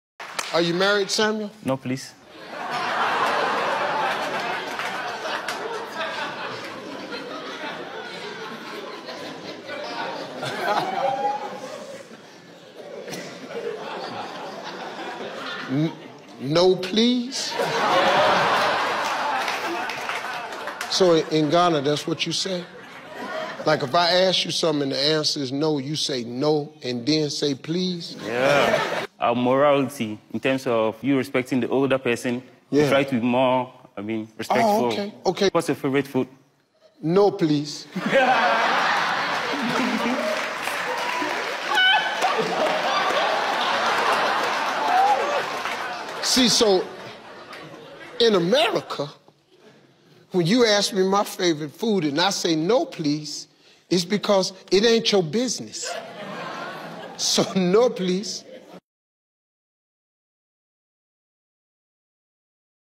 No, Please 🤣🤣🤣
An African guy answering a question from American Man 🤣🤣